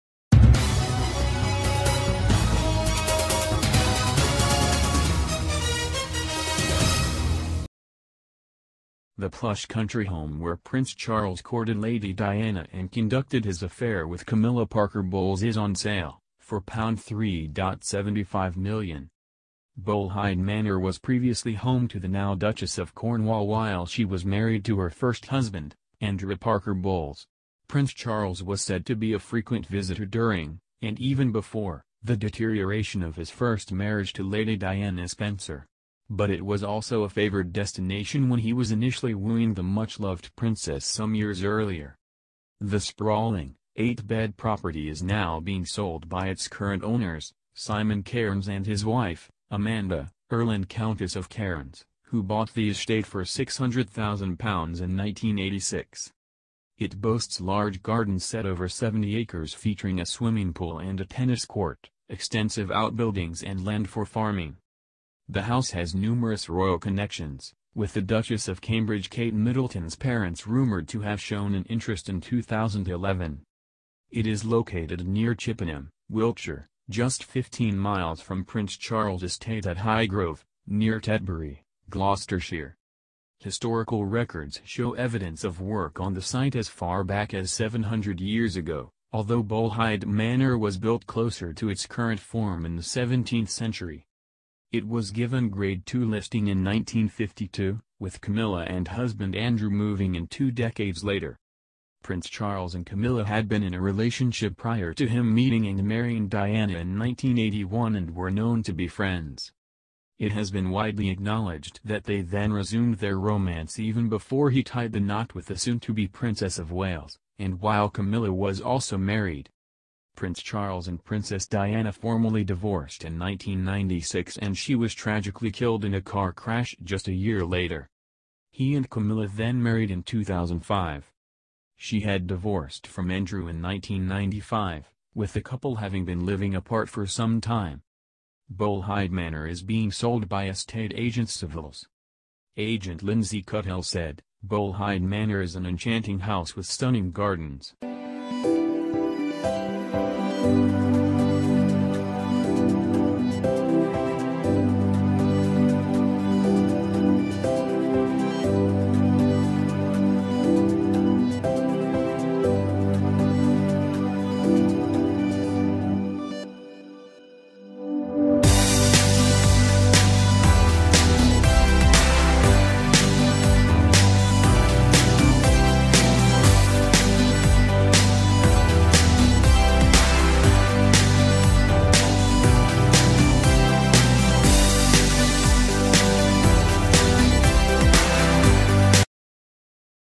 Inside the £3 75m home Prince Charles courted Princess Diana and had affair with Camilla
Harry and William resent Camilla's "disrespectful" act with their deceased mother - what would Camilla be like if william was king?